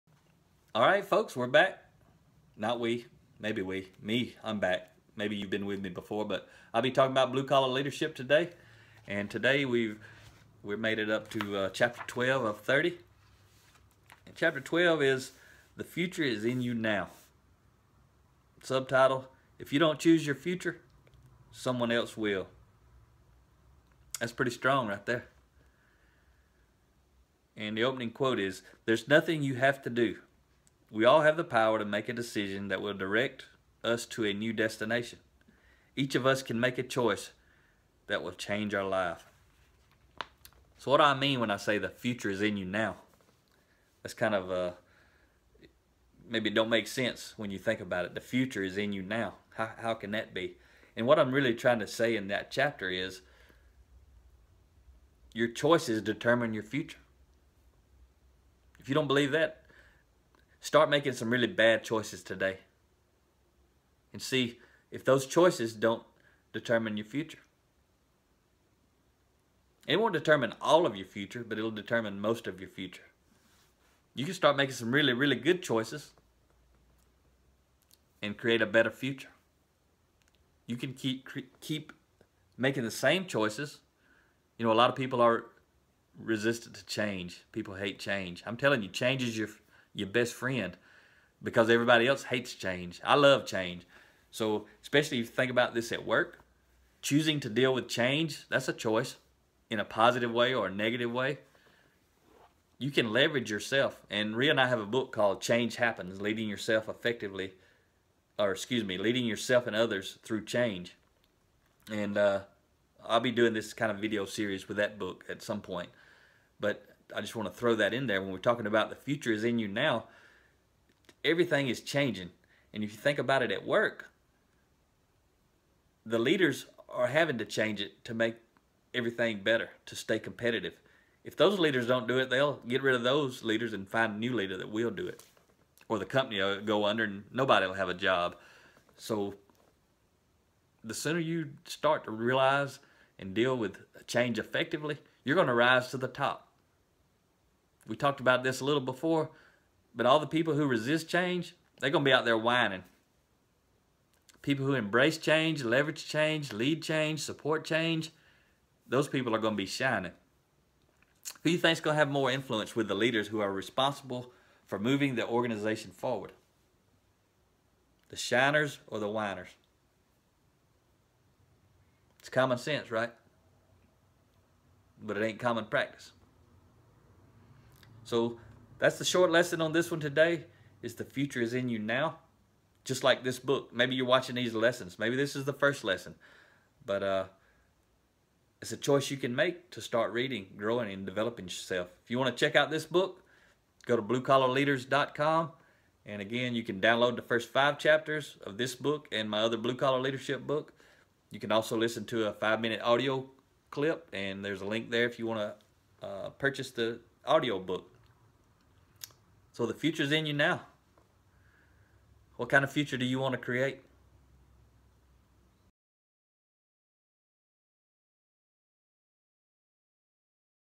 Blue-Collar Leadership (Part 12 of 30)
Unscripted thoughts on Ch.12: THE FUTURE IS IN YOU NOW...If you don't choose your future, someone else will.